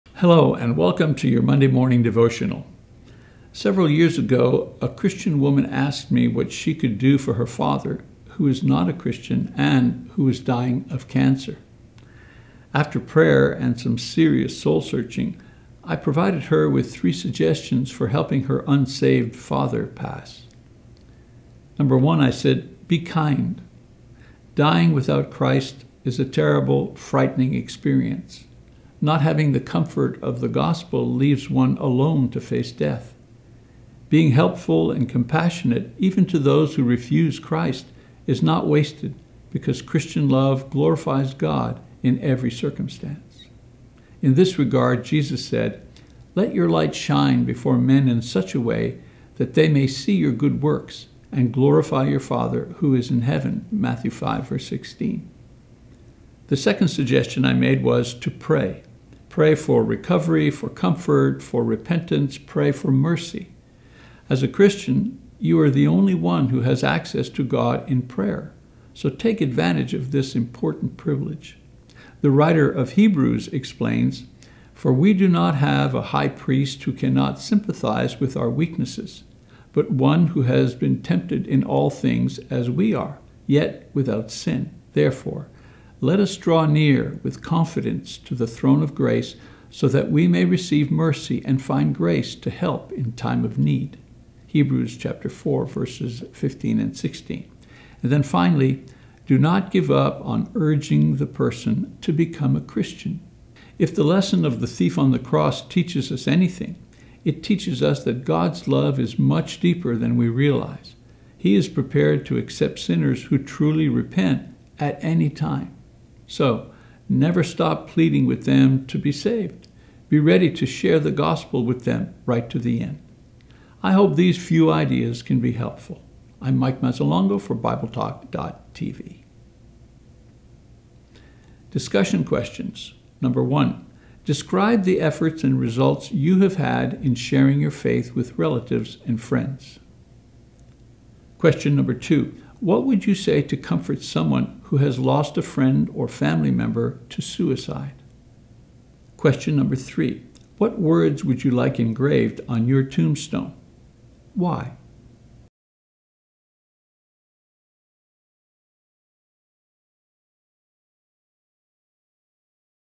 How to Help the Unsaved Die – Mike Mazzalongo | BibleTalk.tv
Several years ago, a Christian woman asked me what she could do for her father, who was not a Christian, and who was dying of cancer. After prayer, and some serious soul-searching, I provided her with three suggestions for helping her unsaved father pass.